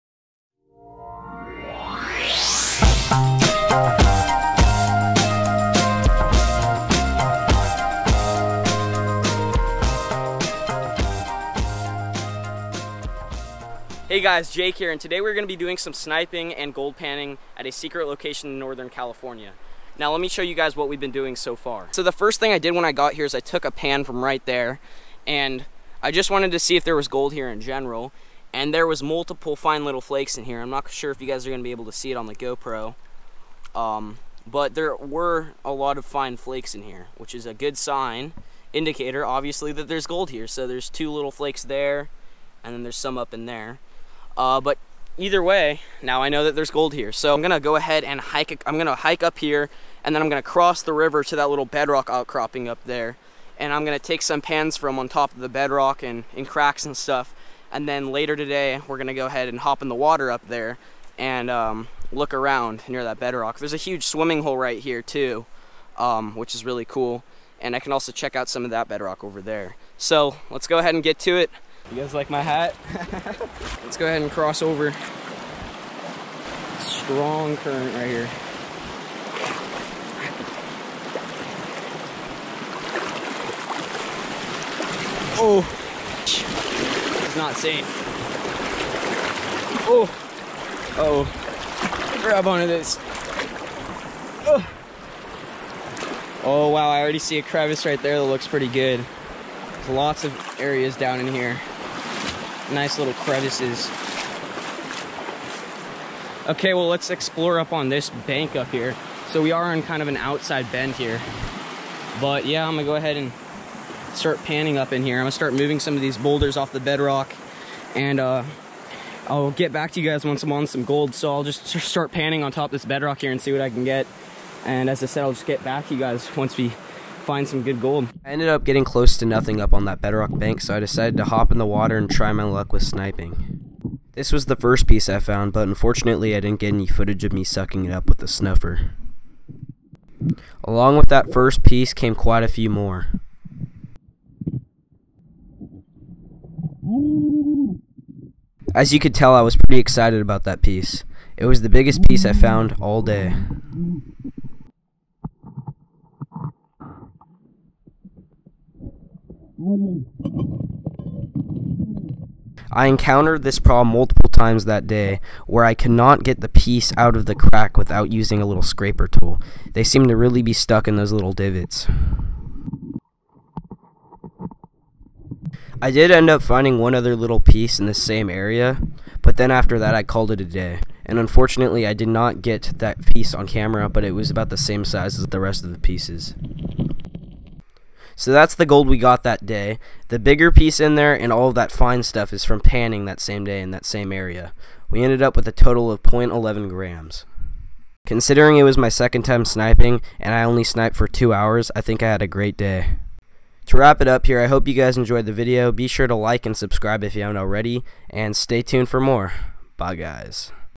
Welcome To Gold Canyon!
By far one of my favorite places to go prospecting ever... Located deep and remote in the woods of Northern California... Defidently one of the most remote places I've been in my life, the gold there is amazing but it is a dangerous place to be... Did not get the best gold in the video but first pan on this river for me
 thanks, Jake
have no rights to the music ,thanks